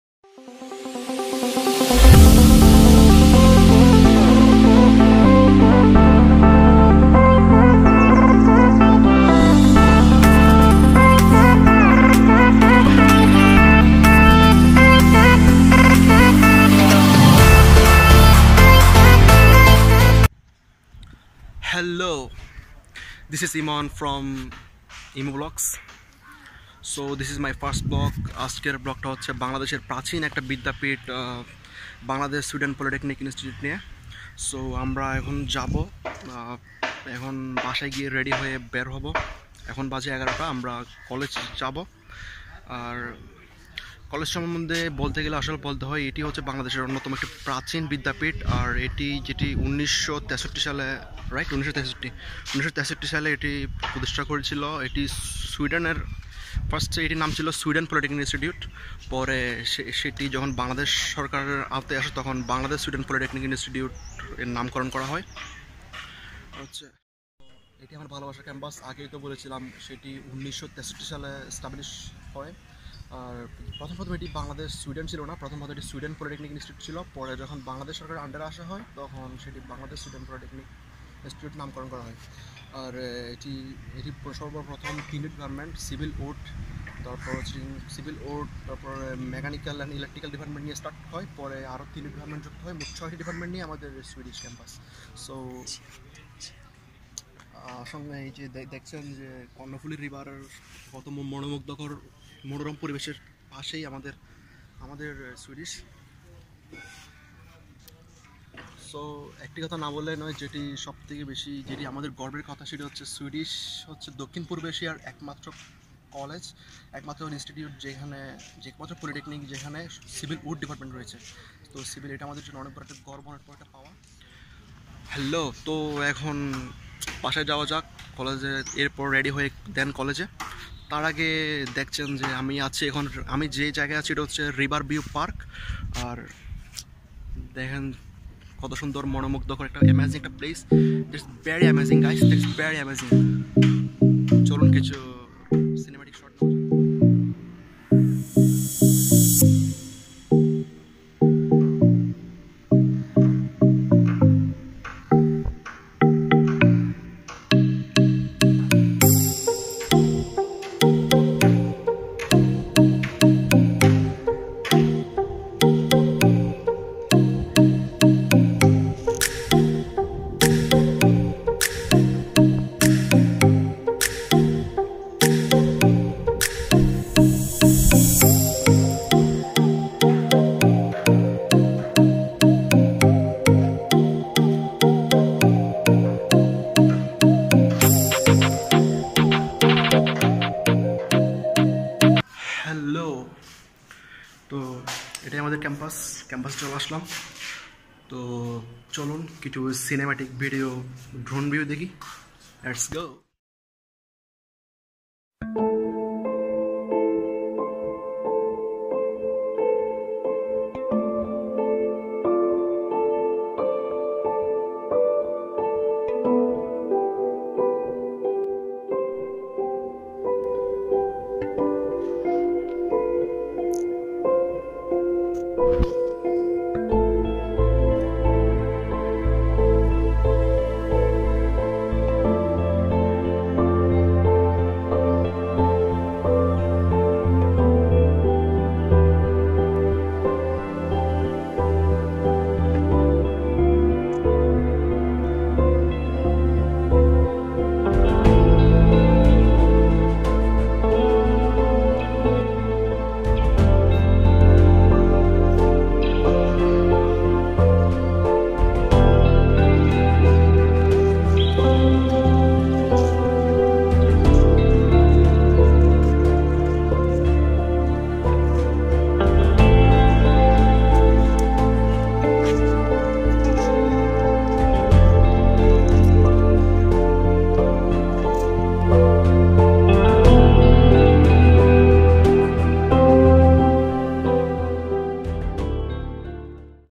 Bangladesh Sweden Polytechnic Instutute || সুইডিস || vlog 1 kaptai || emu vlogs
Bangladeshi sweden polytechnic institute is a historical and traditional institue in Bangladesh.  It's located in Kaptai,  Chittagong.

It has 6 department.  (Computer, Construction,  Electrical, Mechanical, Atumobilr, Civil-Wood)

It's one and only ome of the institute whice have the civil wood Department of whole Asia Sub comtinent... so,  it's really proud for us!

Channel : Emu vlogs
Content Creator : Emon